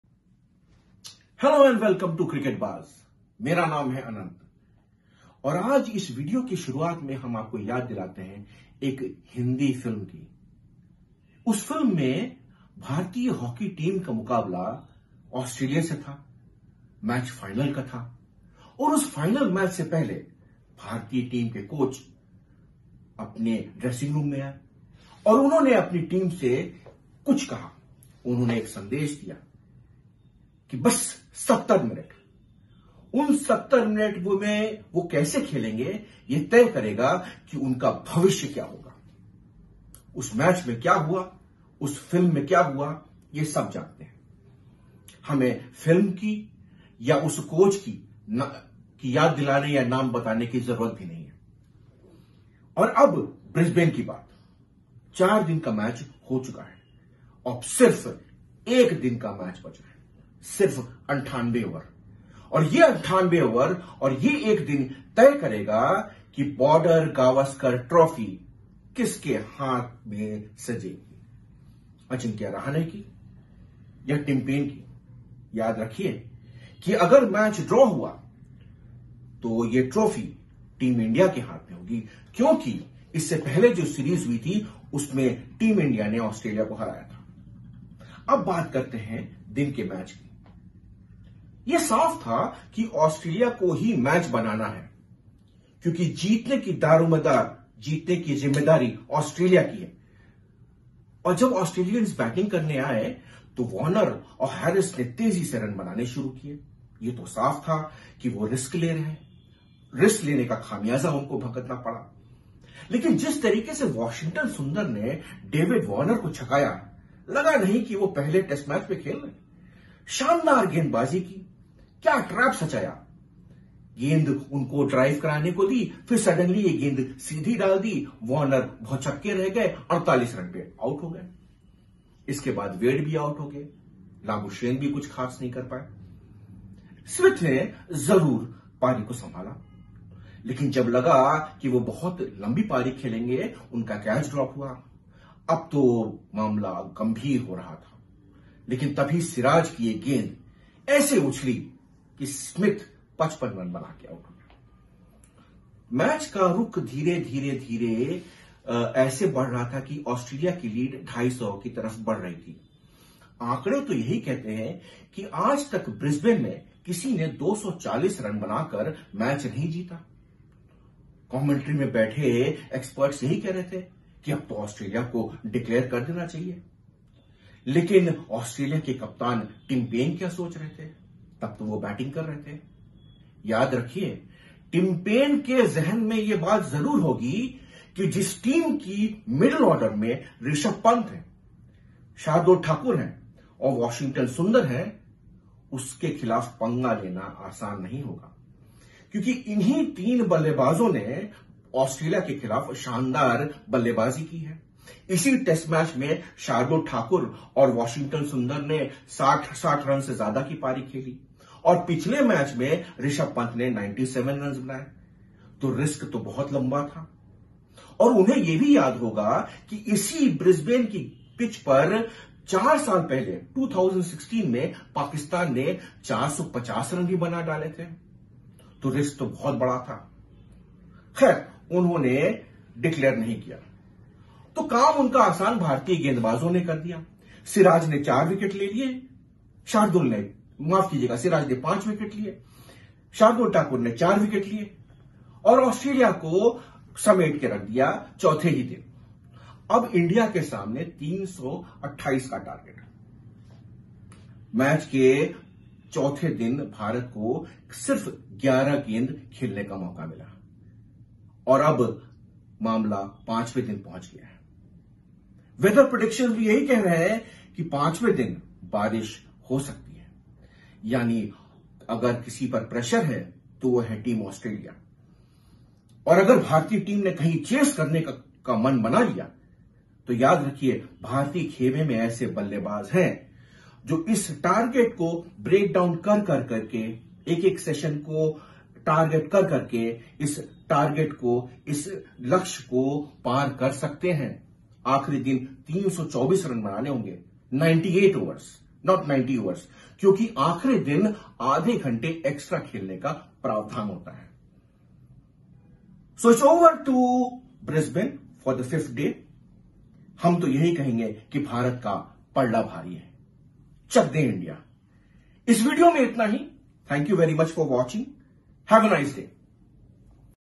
BRISBANE TEST DAY 4 AUSTRALIA VERSUS INDIA REPORT AND ANALYSIS
Team India have the task of batting one full day to ensure that the visitors retain the BorderGavaskarTrophy. India are now 324 runs behind hosts Australia with a possible 98 overs of play remaining. Australia will be under immense pressure on the final day as the weather predictions is that there could be more rain in Brisbane on Tuesday. On the fourth day Mohammad Siraj picked up his first five wicket haul in test matches, while Shardul Thakur bagged 4 wickets. For Australia Steve Smith was the top scorer with 55 runs.